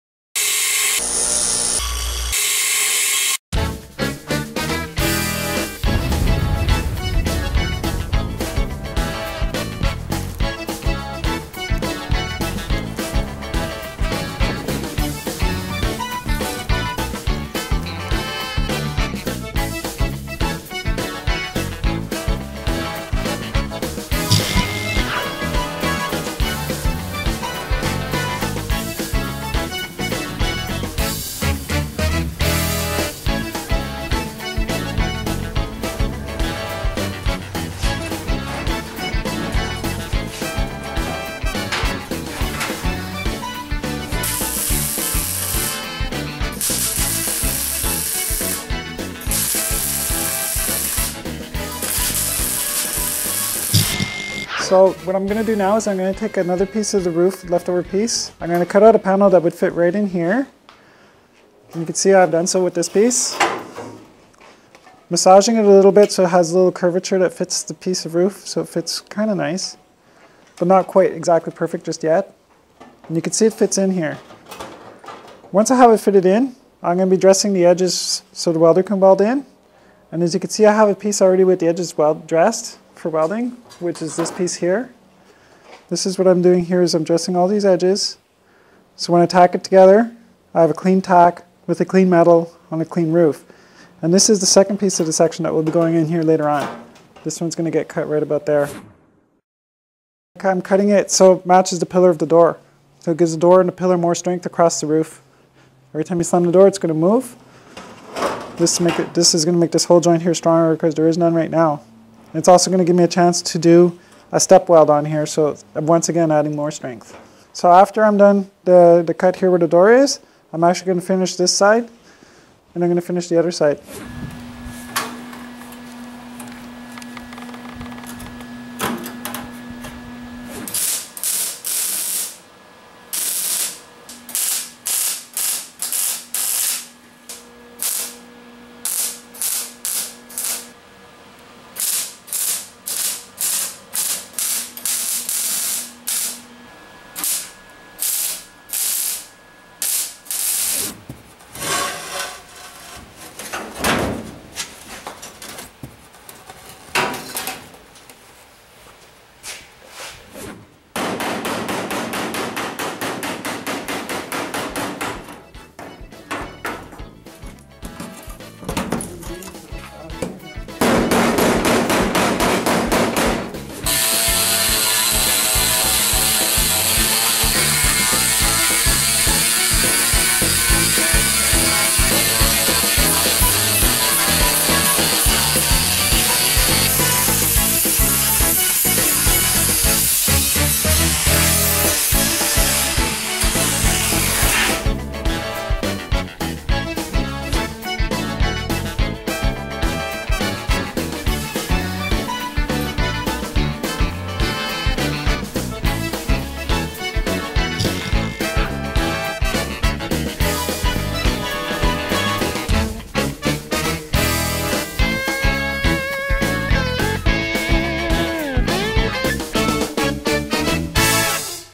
LA Rat Rod Build Episode 6 Roof Side Panels
Rachel adds the side panels to the roof. She tacks them into place but also uses a stitch weld to give the rough finished look.